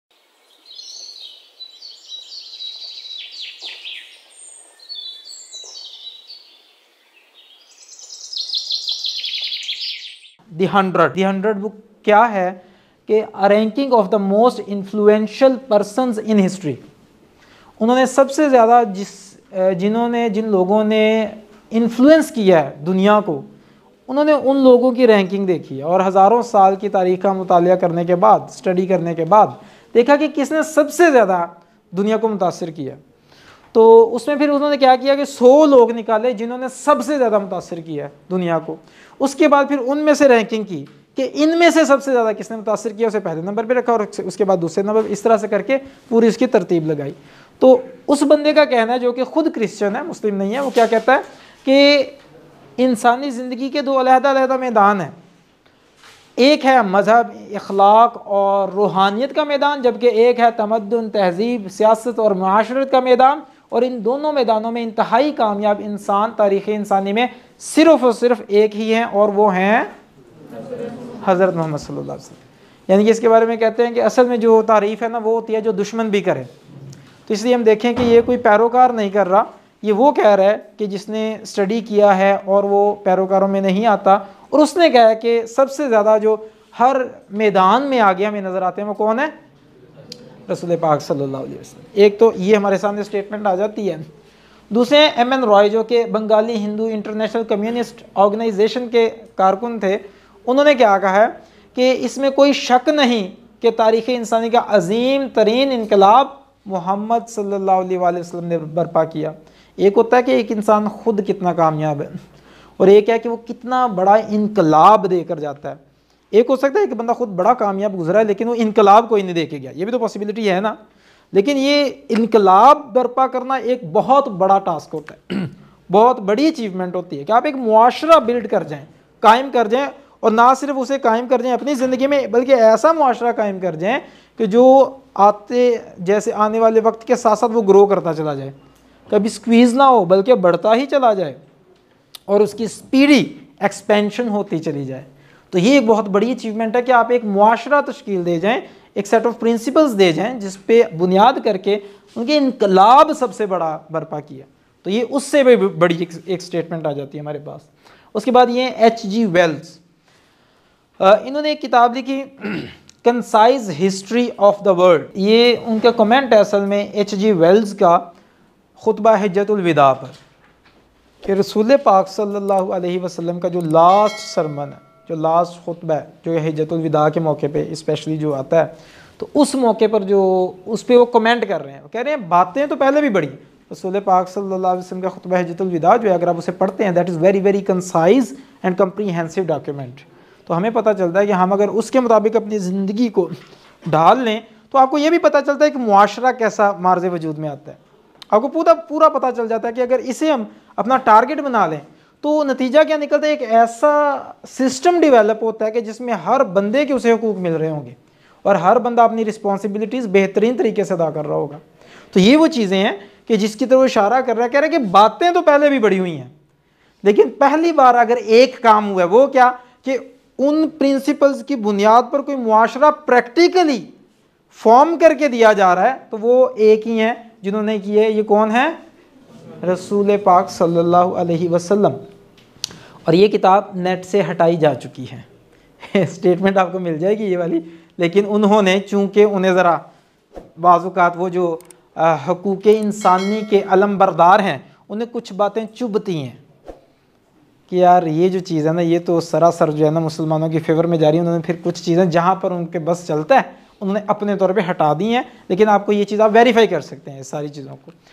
Most Successful Person of Humanity ||Ustad Moin Akhtar
Ustad Moin Akhtar explains about the Most Successful Person of Humanity with reference to Distant Historians at Qasim Ali Shah Foundation